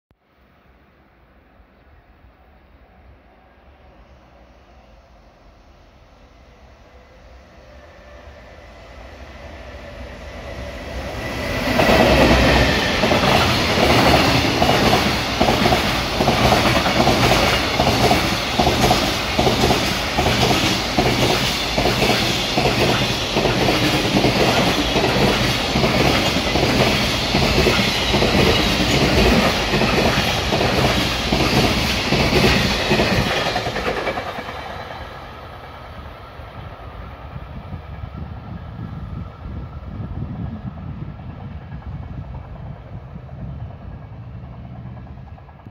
JR貨物 琵琶湖線 貨物列車
近江鉄道 ひこね芹川駅にて撮影